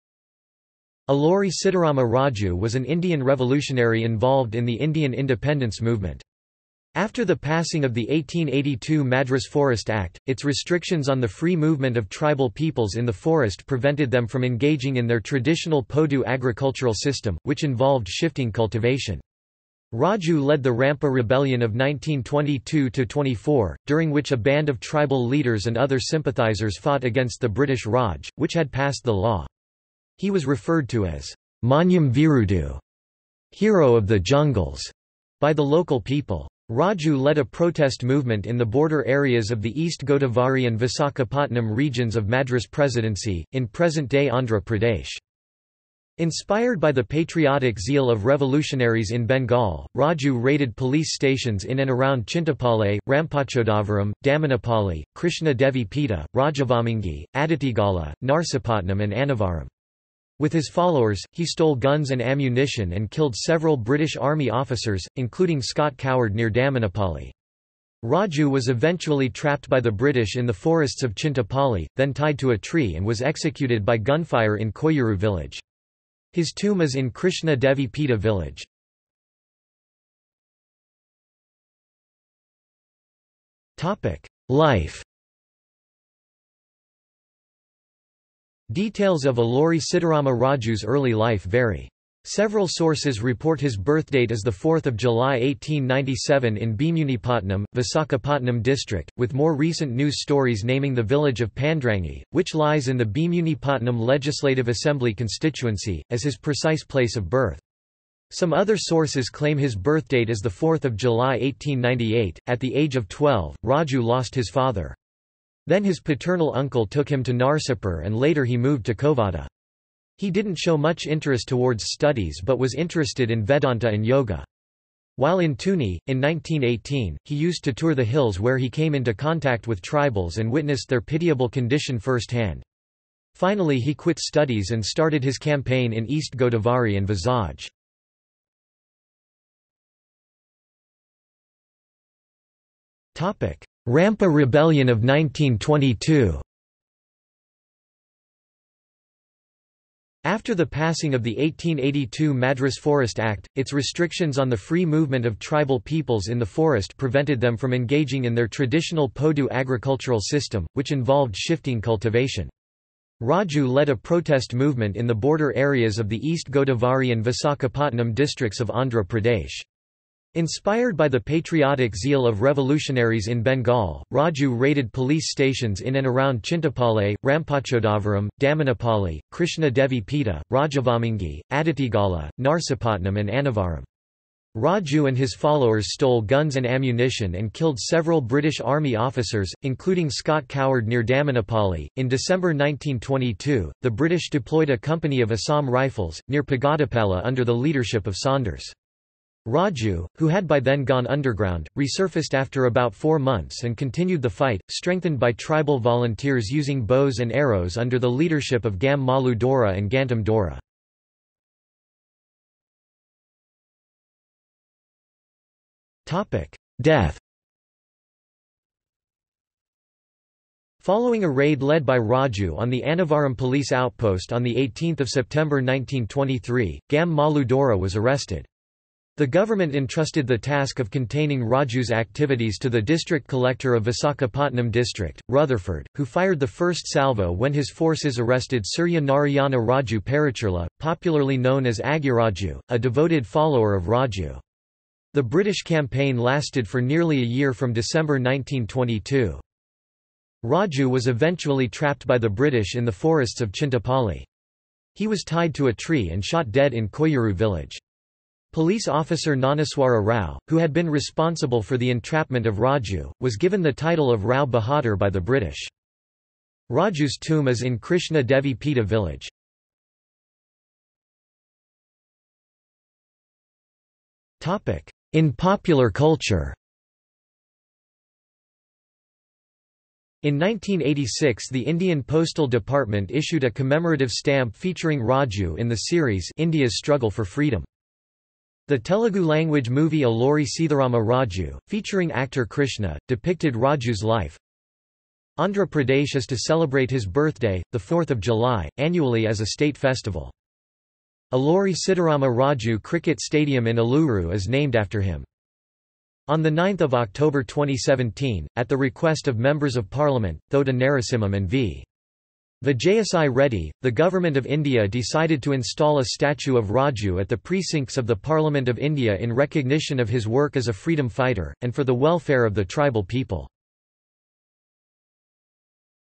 Alluri Sitarama Raju | Wikipedia audio article
This is an audio version of the Wikipedia Article:
Alluri Sitarama Raju

00:01:20 1 Life
00:02:19 2 Rampa Rebellion of 1922
00:03:32 3 Death
00:04:30 4 In popular culture

- Socrates

SUMMARY
Alluri Sitarama Raju was an Indian revolutionary involved in the Indian independence movement. After the passing of the 1882 Madras Forest Act, its restrictions on the free movement of tribal peoples in the forest prevented them from engaging in their traditional podu agricultural system, which involved shifting cultivation. Raju led the Rampa Rebellion of 1922–24, during which a band of tribal leaders and other sympathisers fought against the British Raj, which had passed the law. He was referred to as "Manyam Veerudu" ("Hero of the Jungles") by the local people. Raju led a protest movement in the border areas of the East Godavari and Visakhapatnam regions of Madras Presidency, in present-day Andhra Pradesh.
Inspired by the patriotic zeal of revolutionaries in Bengal, Raju raided police stations in and around Chintapalle, Rampachodavaram, Dammanapalli, Krishna Devi Peta, Rajavommangi, Addateegala, Narsipatnam and Annavaram. With his followers, he stole guns and ammunition and killed several British army officers, including Scott Coward near Dammanapalli. Raju was eventually trapped by the British in the forests of Chintapalli, then tied to a tree and was executed by gunfire in Koyyuru village. His tomb is in Krishna Devi Peta village.